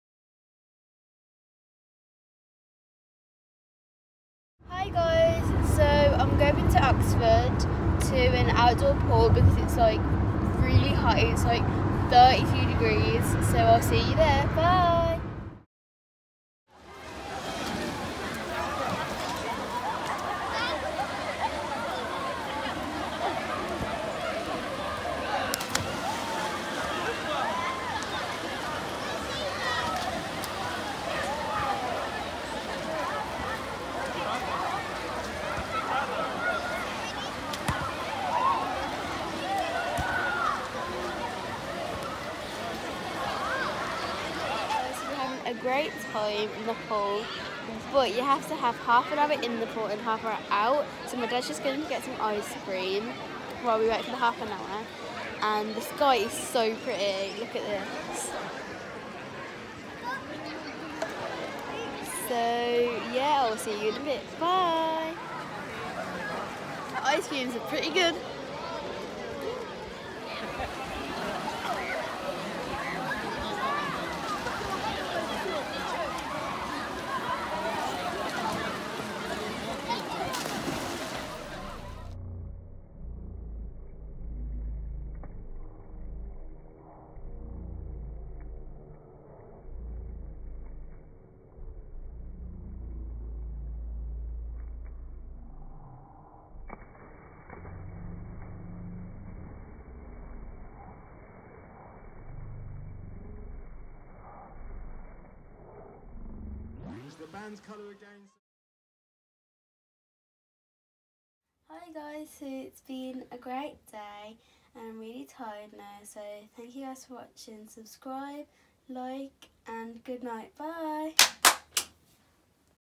Hinksey Outdoor Pool
A great day out in Oxford at Hinksey Outdoor Pool.